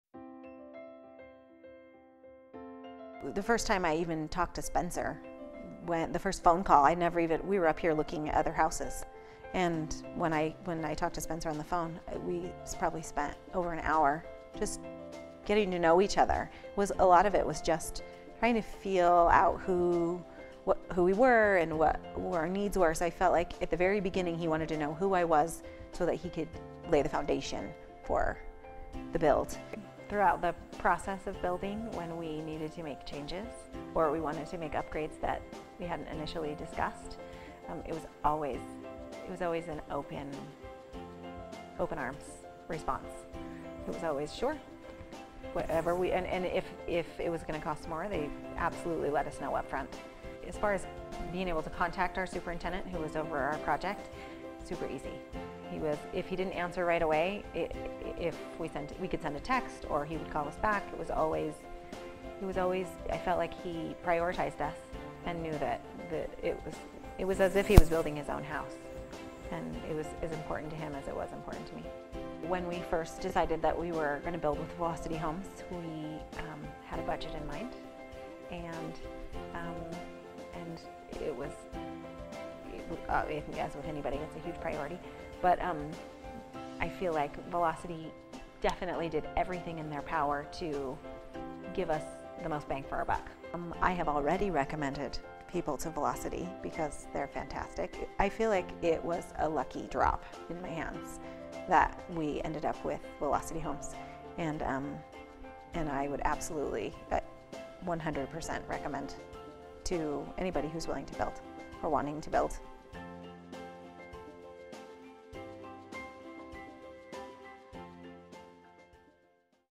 Velocity Homes - Customer Experience
Velocity Homes customer shares her experience working with Velocity Homes in building her custom home.